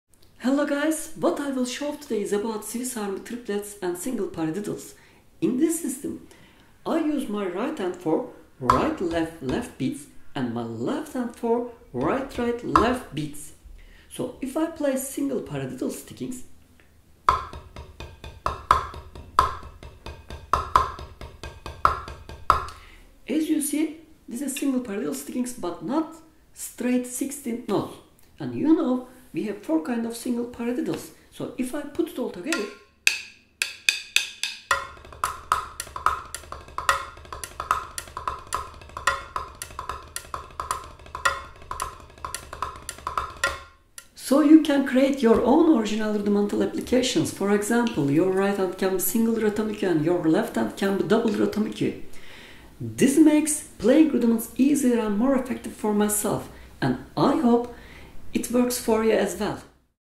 Drum Lesson: How to Play Swiss Army Triplets / How to Practice Rudiments More Effectively
Drum Lesson: How to Play Swiss Army Triplets
How to Practice Rudiments More Effectively